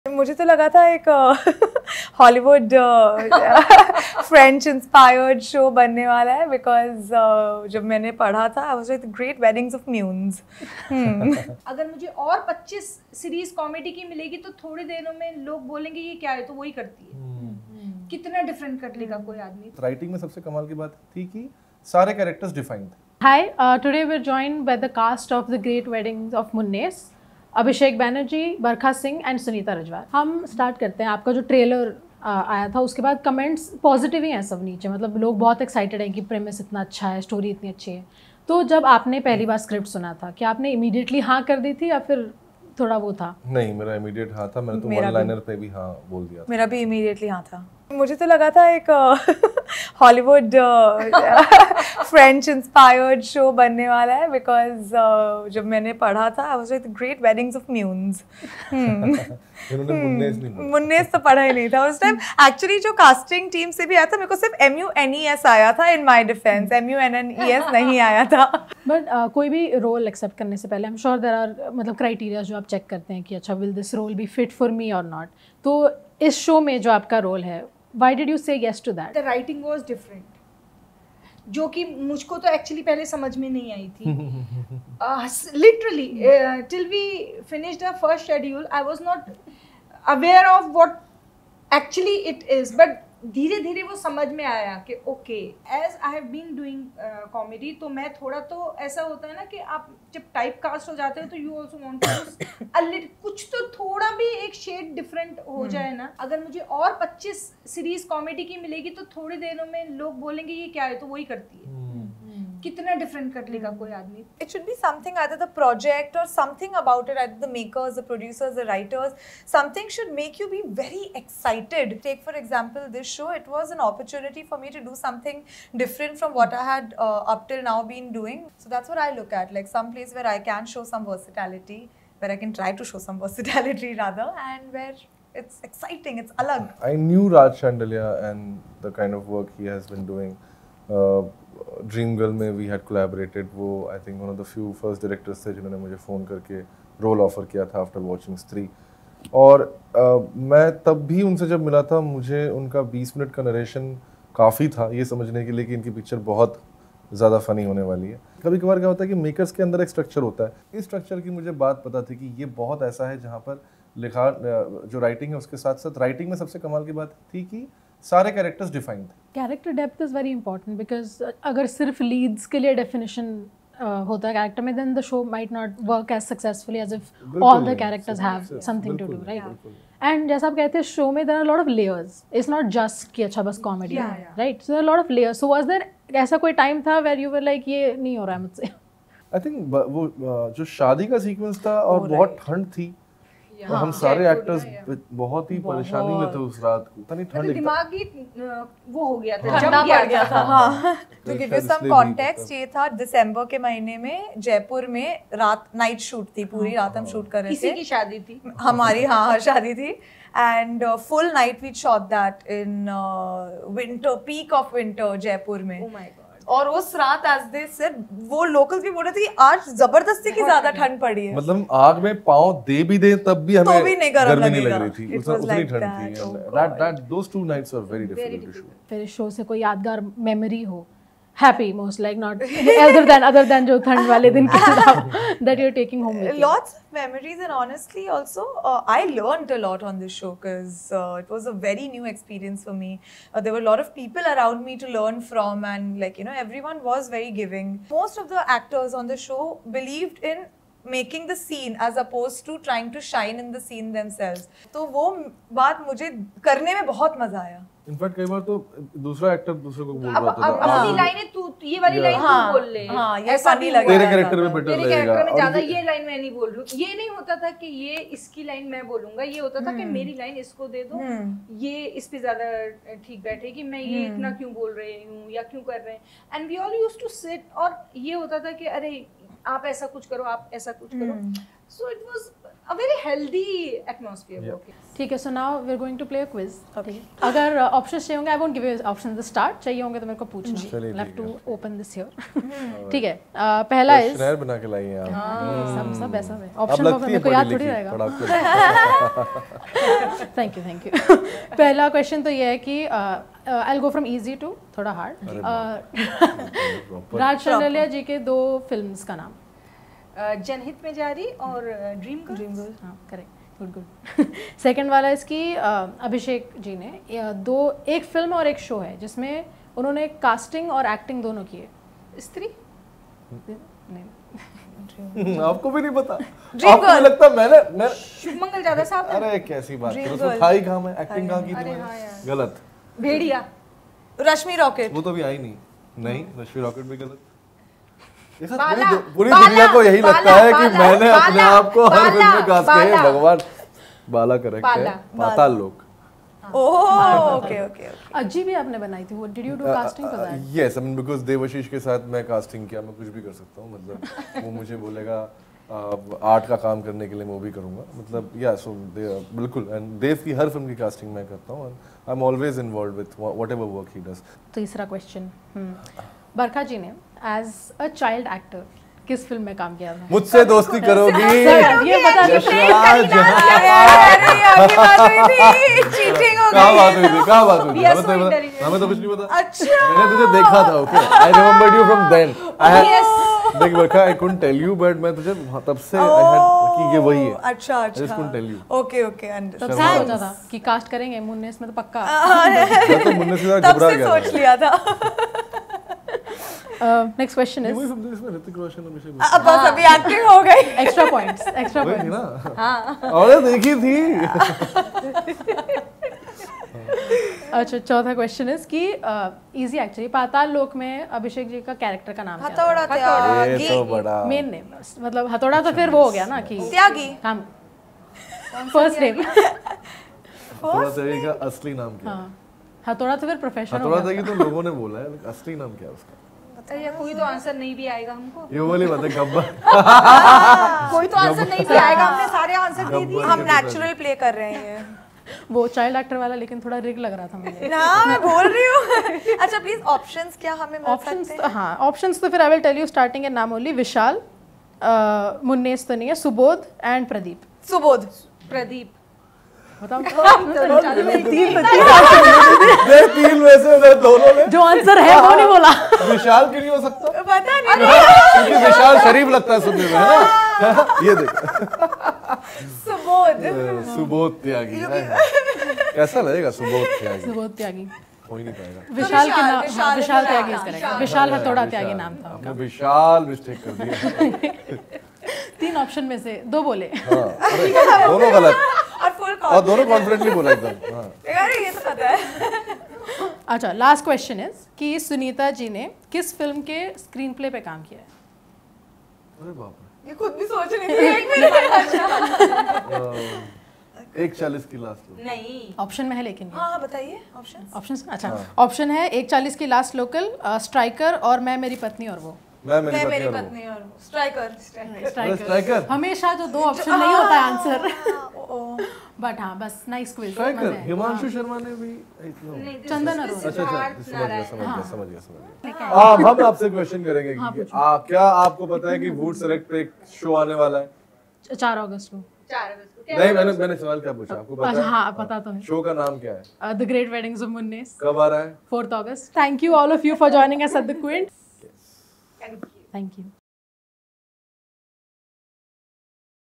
Abhishek Banerjee, Barkha Singh & Sunita Rajwar on How They Choose Roles & Being Typecast| The Quint
TheGreatWeddingsofMunnes stars AbhishekBanerjee, BarkhaSingh, and SunitaRajwar talk about their show, their roles, being typecast, their experiences on set and more.